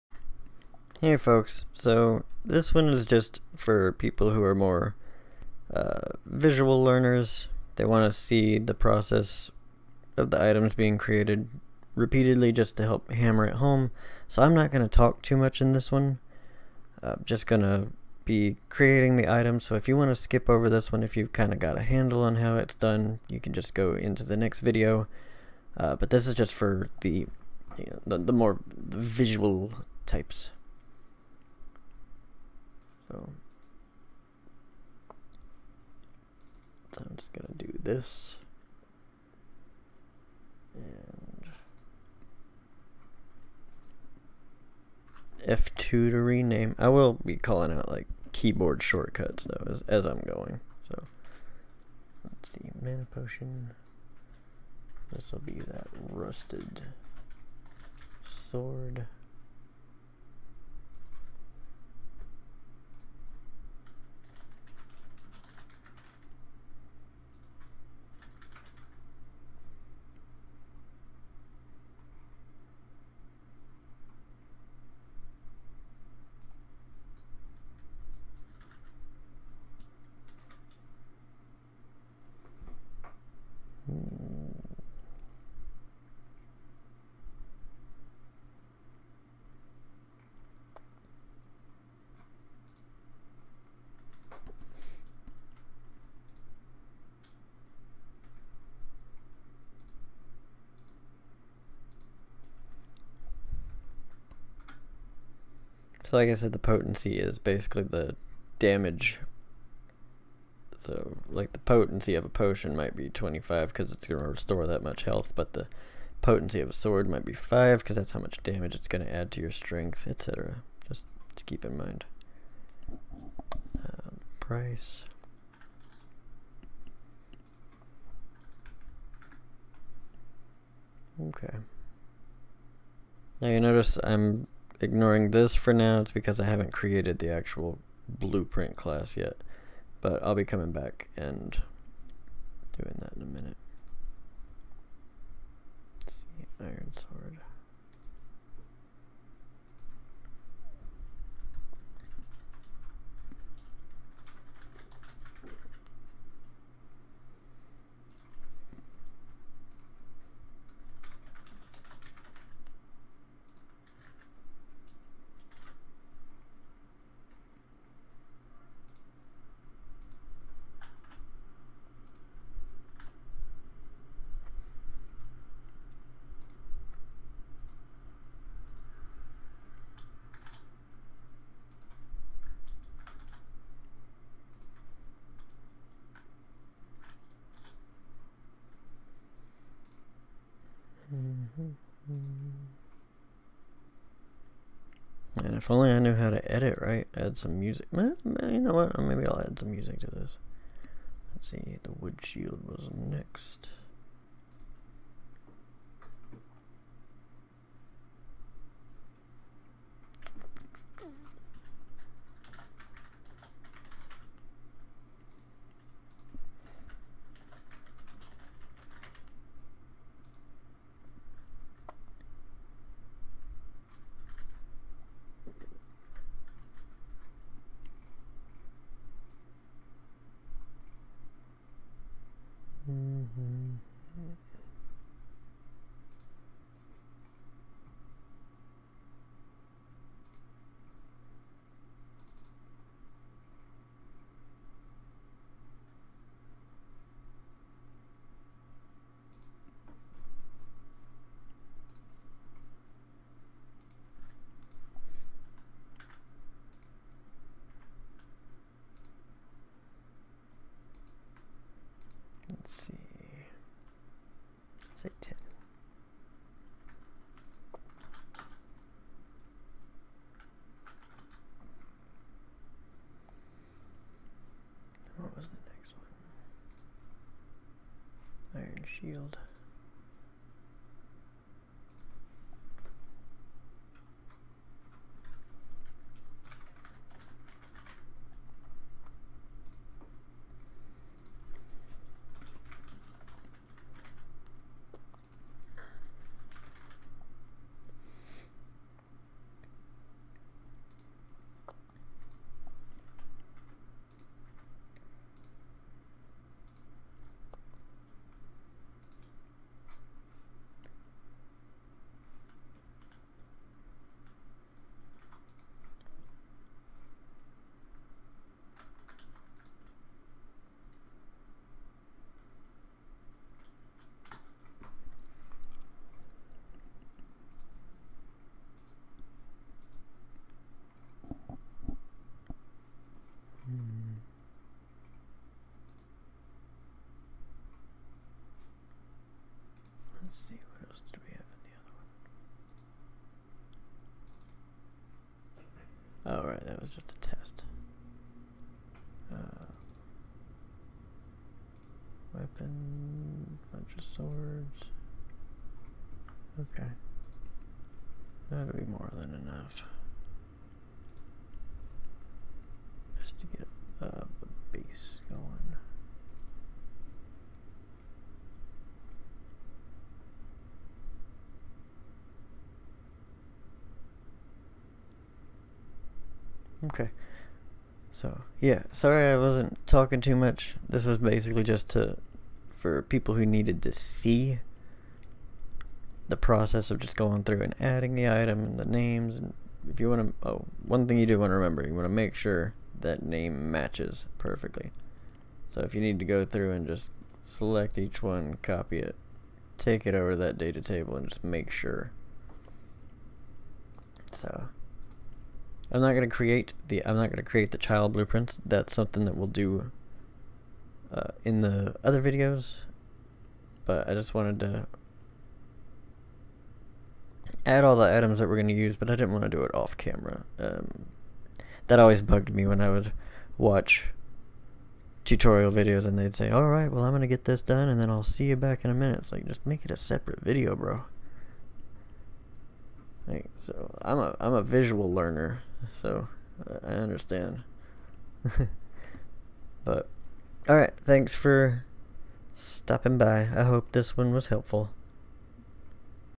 Unreal Engine 5 RPG Tutorial Series #5.5: Inventory system part 2.5, Populating Data Table
This video is just for people that want to see the process of adding items to the data table a few extra times, the more visual learners that like to see the stuff to help hammer it home (like me XD) so if you've already added your items to your list, feel free to skip